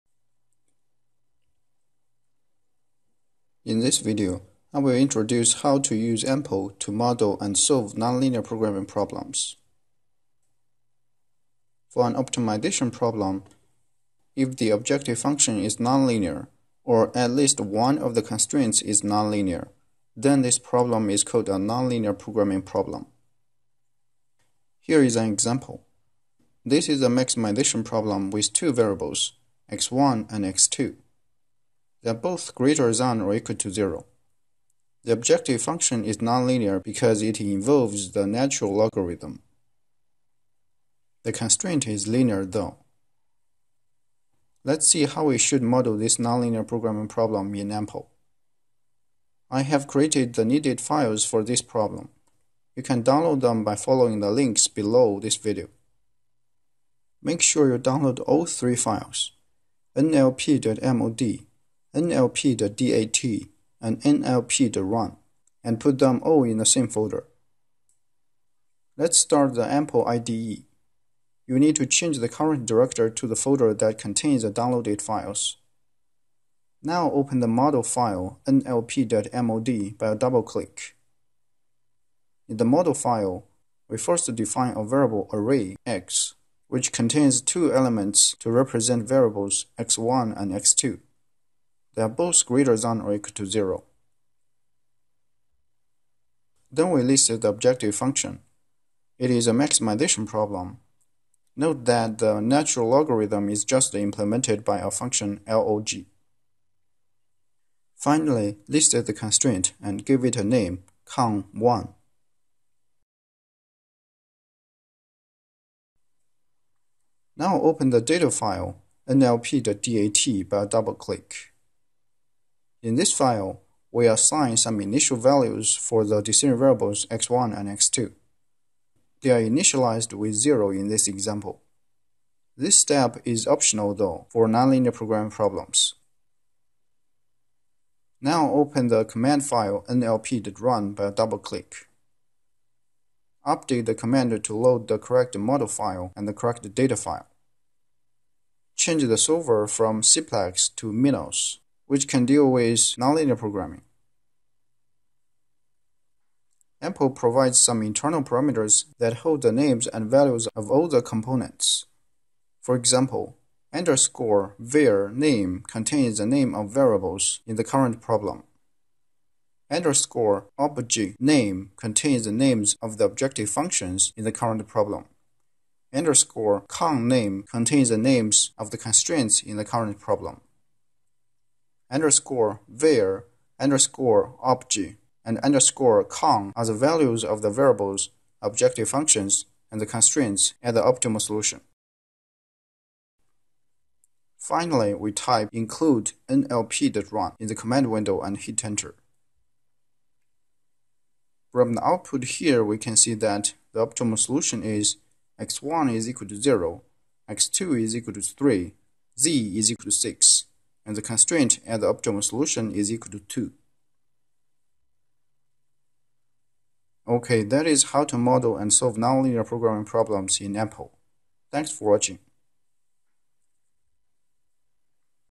Operations Research 15E: AMPL - Nonlinear Programming
In this video, I'll introduce how to use AMPL to model and solve nonlinear programming problems.